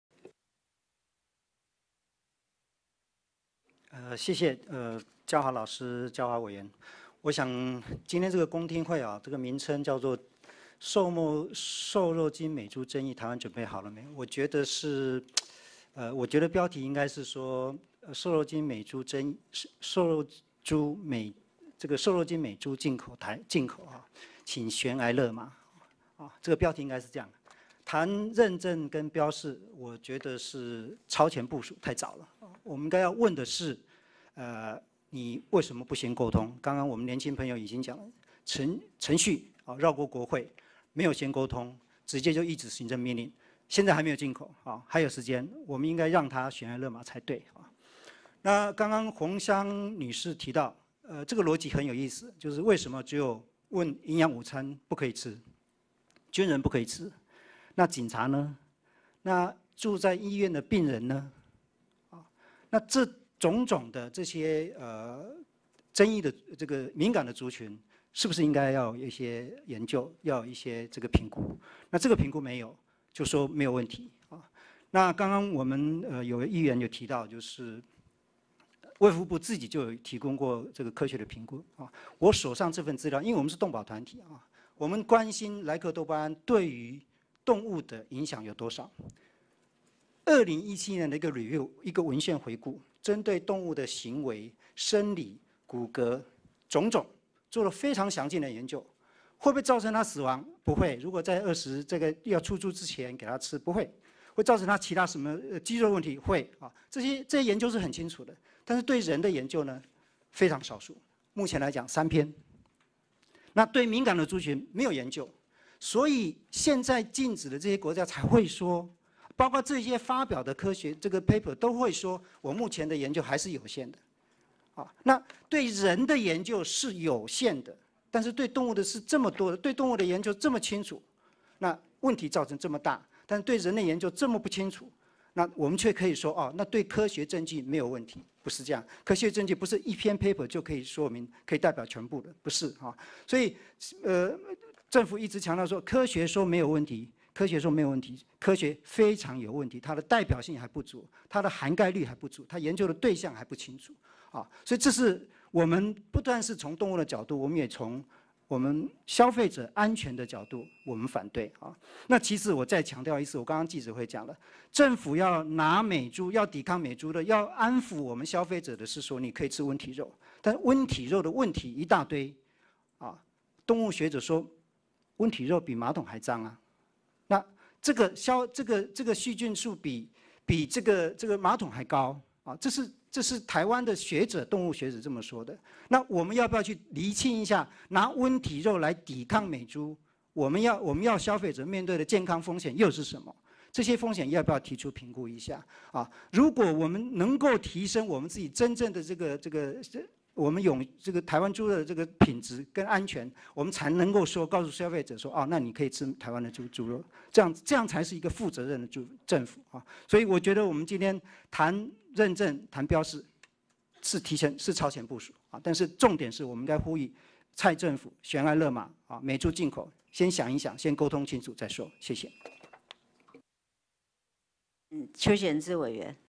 20200903 台灣動物社會研究會執行長 朱增宏 發言 「進口肉品原產地認證與加工品標示管制」公聽會
20200903  「進口肉品原產地認證與加工品標示管制」公聽會 台灣動物社會研究會執行長 朱增宏 發言